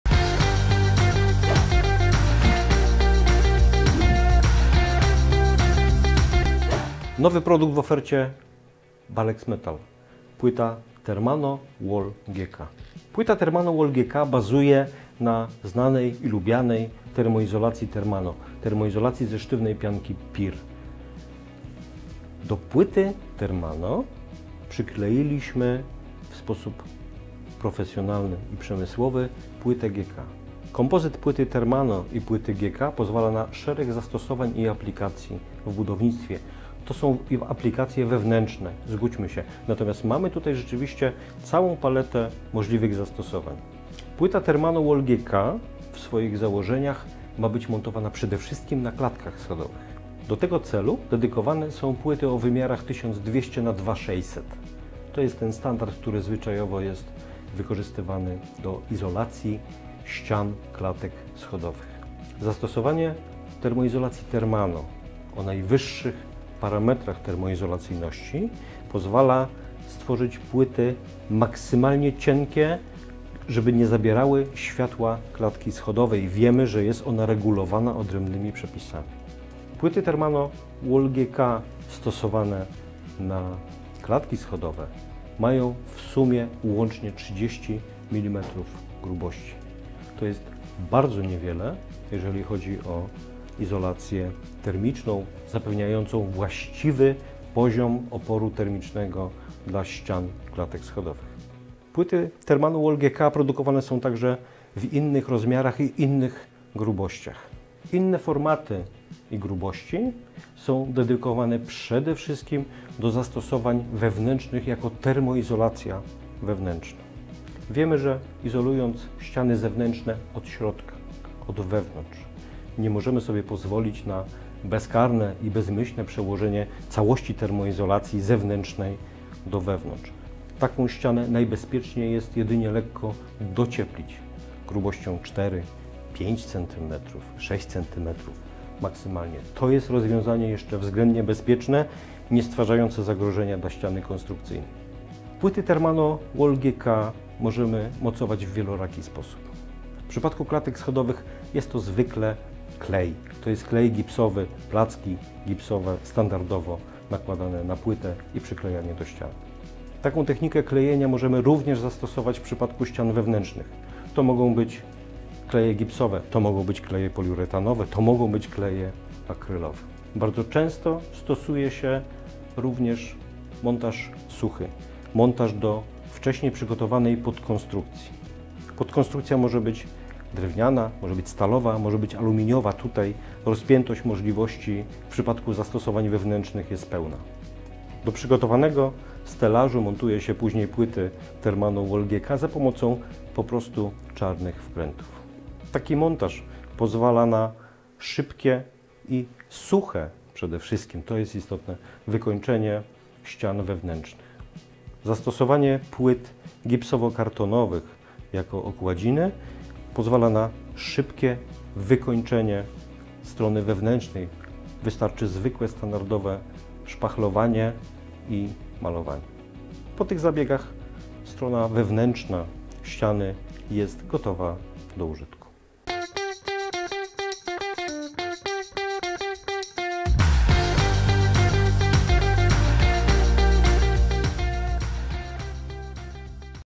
Ocieplenie domu od wewnątrz - THERMANO GK.
Przeskocz do części, która Cię interesuje

0:00 Intro
0:12 Budowa produktu do izolacji ścian i sufitów od wewnątrz Thermano GK.
0:44 Zastosowanie - montaż na klatkach schodowych.
1:41 Rozmiary i grubości płyt termoizolacyjnych Thermano GK.
1:57 Jaką maksymalną grubość docieplenia zastosować dla ścian izolowanych od wewnątrz?
2:25 Sposoby montażu płyt izolacyjnych Thermano GK na ścianach i sufitach od środka.

Jak docieplić dom od wewnątrz?

Płyta Thermano GK bazuje na doskonale znanej termoizolacji Thermano, czyli termoizolacji ze sztywnej pianki PIR. Natomiast w tej odsłonie, do płyty Thermano przykleiliśmy w sposób przemysłowy okładzinę GK.

Kompozyt płyt Thermano i GK pozwala na szereg zastosowań i aplikacji w budownictwie przy czym należy podkreślić, że są to aplikacje wewnętrzne. Płyta Thermano GK może być montowana na klatkach schodowych. Do tego celu są przygotowane płyty o wymiarach 1200x2600 mm, najczęściej stosowane do izolacji ścian klatek schodowych. Zastosowanie izolacji Thermano o najwyższych parametrach izolacyjności λ (lambda)= 0,023 (W/mK) pozwala nam dostarczać płyty maksymalnie cienkie, tak aby nie zabierały światła klatki schodowej ponieważ minimalna, dopuszczalna przestrzeń klatek schodowych jest regulowana odrębnymi przepisami. Płyty izolacyjne z okładziną GK stosowane na klatki schodowe mają łącznie 30 mm grubości. To jest bardzo niewiele jeżeli chodzi o izolację termiczną zapewniającą właściwy poziom oporu termicznego dla ścian klatek schodowych.

Płyty Thermano GK są oczywiście produkowane również w innych rozmiarach (1200x2600 mm, 1200x600 mm) i grubościach (30, 50, 60, 120 mm), a to dlatego aby móc zastosować je jako wewnętrzną termoizolację. Należy jednak pamiętać, że izolując ściany zewnętrzne od środka, nie można przełożyć całości termoizolacji zewnętrznej do wewnątrz. Taką ścianę jest najlepiej lekko docieplić grubością 4-6 cm. Takie rozwiązanie jest bezpieczne i nie stwarzają zagrożenia dla ściany konstrukcyjnej.

Płyty Thermano GK daje kilka opcji mocowania. W przypadku klatek schodowych najczęściej wykorzystuje się do tego klej gipsowy - placki kleju standardowo są nakładane na płytę, a następnie przyklejane do ściany. Taką technikę klejenia można również stosować w przypadku ścian wewnętrznych wykorzystując do tego kleje gipsowe, poliuretanowe lub akrylowe. Bardzo często wykorzystuje się również montaż suchy, czyli montaż do przygotowanej podkonstrukcji drewnianej, stalowej lub aluminiowej. Do przygotowanego stelażu płyty są mocowane za pomocą czarnych wkrętów. Taki montaż pozwala na szybkie i suche wykończenie ścian wewnętrznych. Bardzo zachęcamy wykorzystanie tej możliwości, jaką daje Thermano GK i mocowanie płyty w sposób mechaniczny. Dzięki temu oszczędzamy wodę, której ograniczanie do celów budowlanych z całą mocą wspieramy.

Zastosowanie płyt gipsowo-kartonowych jako okładziny pozwala również na szybkie wykończenie strony wewnętrznej budynku. Wystarczy tylko wyszpachlować powierzchnię i wymalować ściany i sufit. Po tych pracach pomieszczenie jest gotowe do użytku.

Jeśli jesteście zainteresowani rozwiązaniami z użyciem płyt izolacyjnych Thermano GK, koniecznie sprawdźcie szczegółowe informacje o tym produkcie